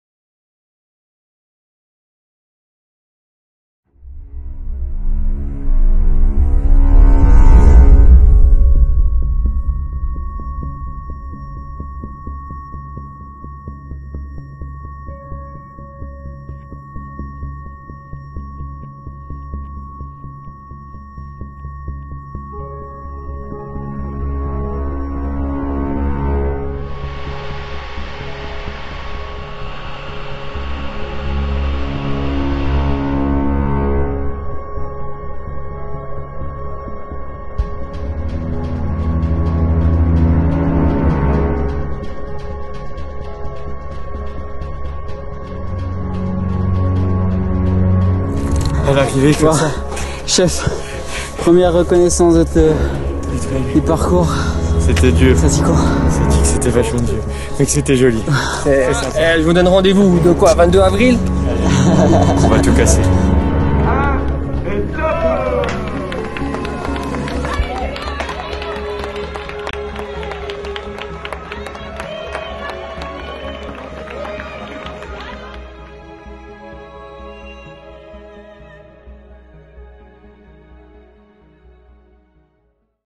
Teaser Trail N Loue 2018 by Clara
Au coeur de la haute vallée de la Loue, le village e Mouthier-Haute-Pierre sera le coeur de la cinquième édition du Trail N Loue le 21 et 22 Avril 2018. Quatre parcours au programme (12km/18km/32km) et la nouveauté : une course sur deux jours en équipe de deux !